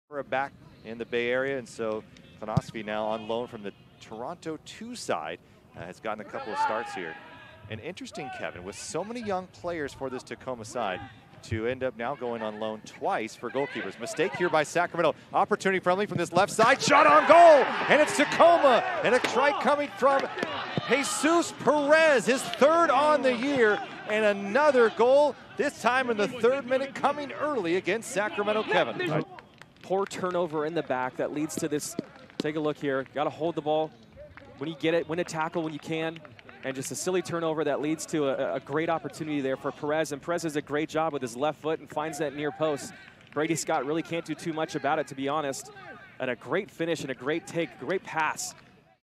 Jesús Pérez with a Goal vs. Sacramento Republic FC
Jesús Pérez (Tacoma Defiance) with a Goal vs. Sacramento Republic FC, 09/20/2020